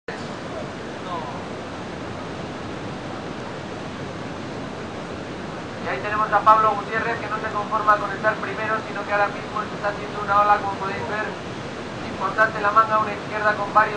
Bennett-Watt Hd Productions Inc Inc
Surfers ride waves in the Atlantic Ocean.